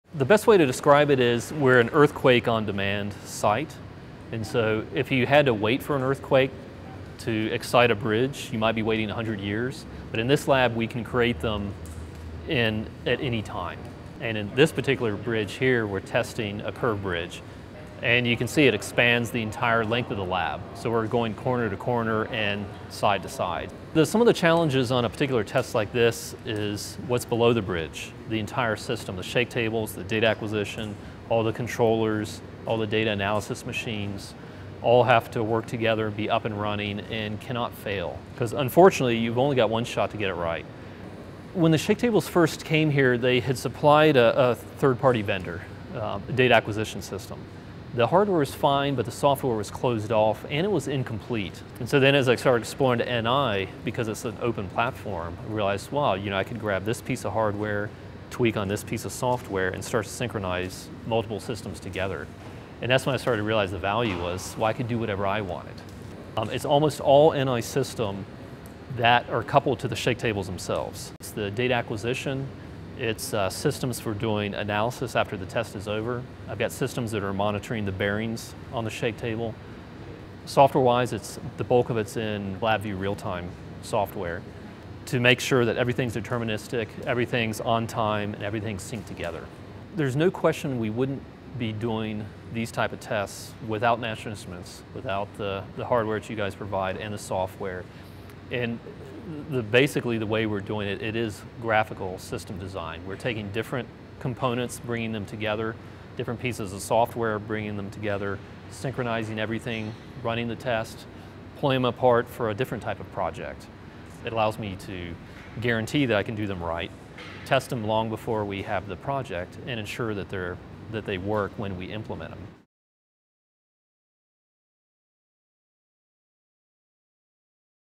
Structural Testing for the Effect of Live Loads on the Seismic Response of Bridges
for more information. Today's bridges are being designed with unique constraints such as live vehicle loads and earthquake loads. However, bridges are not being designed for both of these conditions at the same time. The University of Nevada, Reno was sanctioned by a public agency to study how a vehicle's suspension system interacts with a bridge during an earthquake. To accurately test complex structures like curved bridge sections, the university built large bridge sections that can be tested with large hydraulic shakers capable of simulating earthquakes. Measurements such as strain, force, and displacement were made using PXI hardware and NI LabVIEW software to characterize the behavior of the supporting structures as well as the movement of the bridge and vehicles.